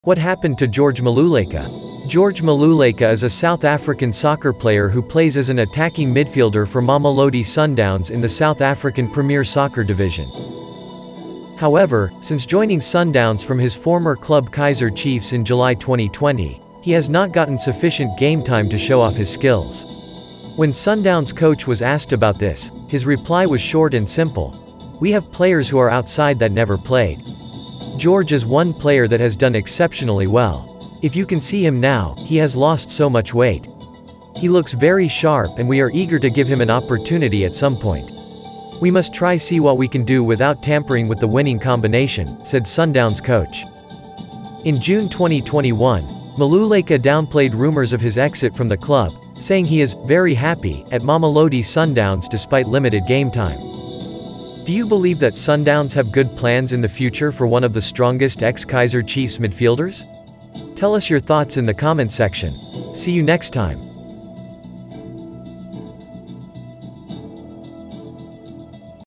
What Happened To George Maluleka? | Sundowns News
What happened to George Maluleka?

George Nyiko Maluleka is a South African soccer player who plays for as an Attacking Midfielder for Mamelodi Sundowns in the South African Premier Soccer Division.

However, since joining Sundowns from his former club Kaizer Chiefs in July 2020, he has not gotten sufficient game time to show off his skills.

When Sundowns coach was asked about this, his reply was short and simple:

"We have players who are outside that never played. George is one player that has done exceptionally well. If you can see him now, he has lost so much weight. He looks very sharp and we are eager to give him an opportunity at some point. We must try see what we can do, without tampering with the winning combination,"
Said Sundowns coach

In June 2021, Maluleka downplayed rumours of his exit from the club, saying he is 'very happy' at Mamelodi Sundowns despite limited game time.

Do you believe Sundown s has good plans for one of the strongest ex Amakhosi midfielder?